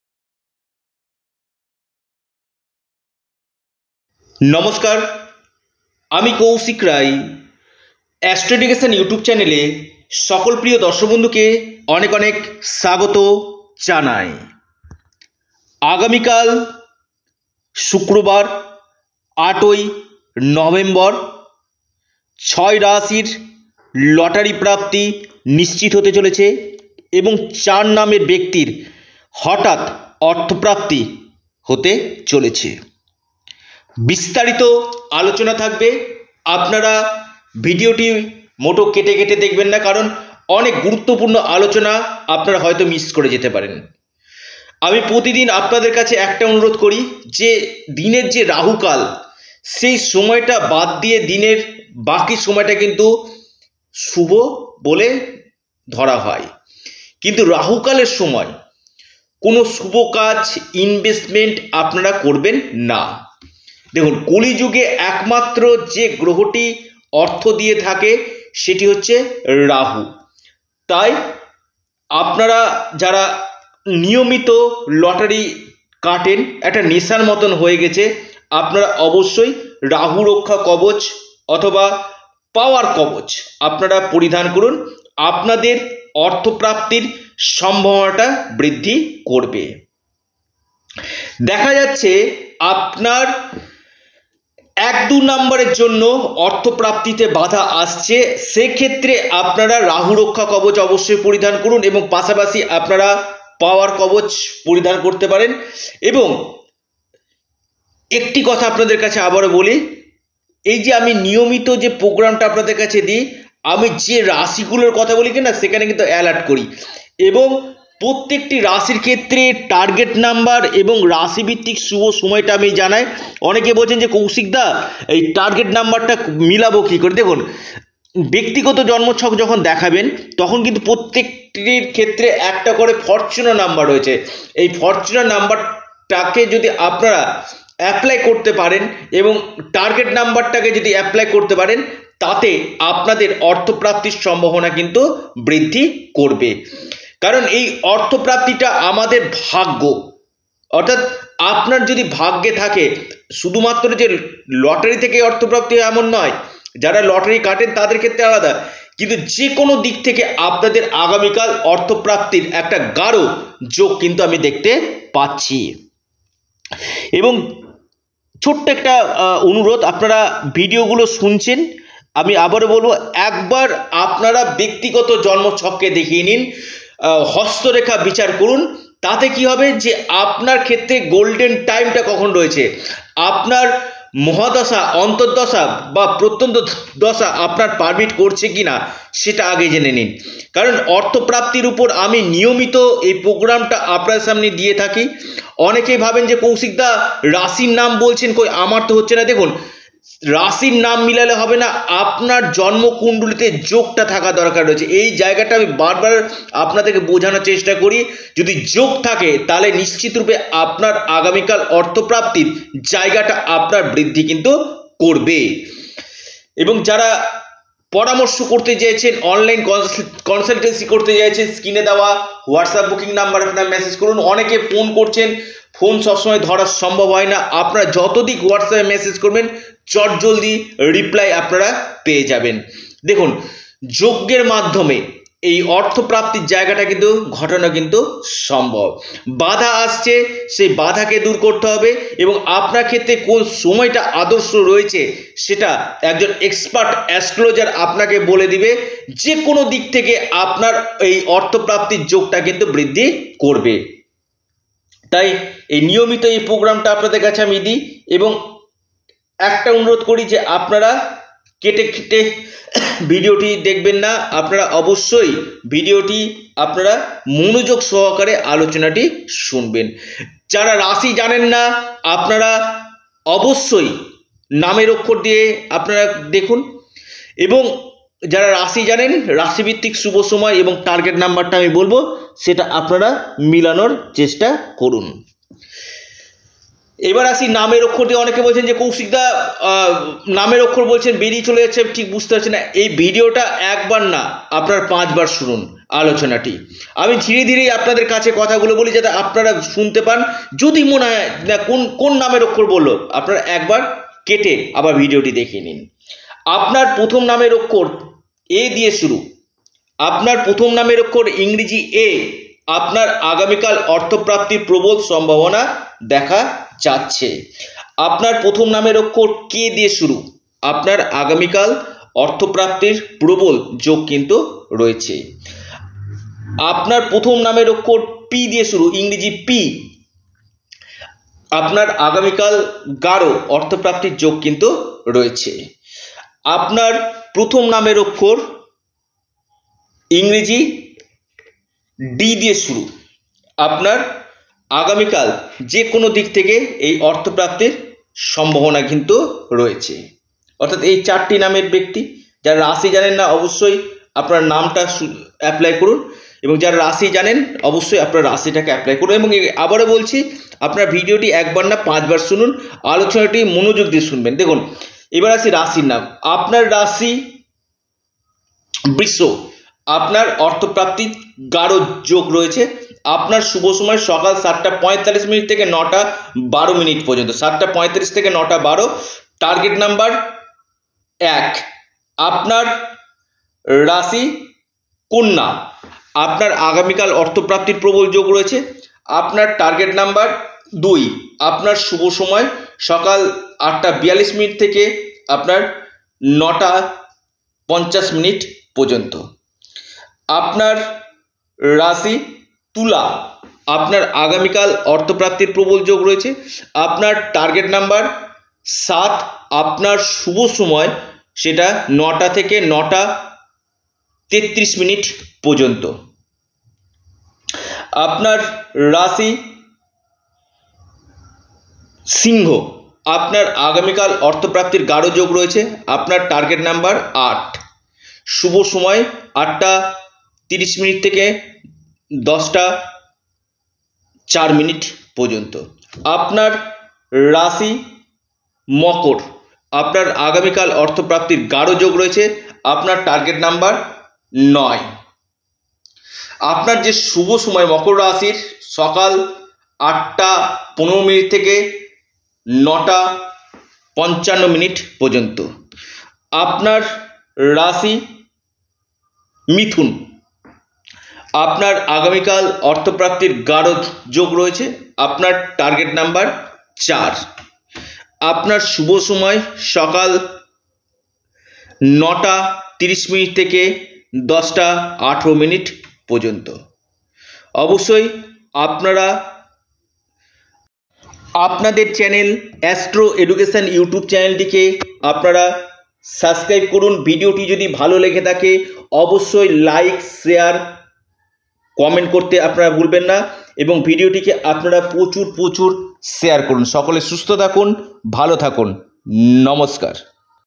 8 November 2024 আগামীকাল শুক্রবার ছয়টি রাশির লটারি প্রাপ্তি নিশ্চিত হতে চলেছে!
আগামীকাল এই ছয়টি রাশির হঠাৎ অর্থ প্রাপ্তি হতে চলেছে।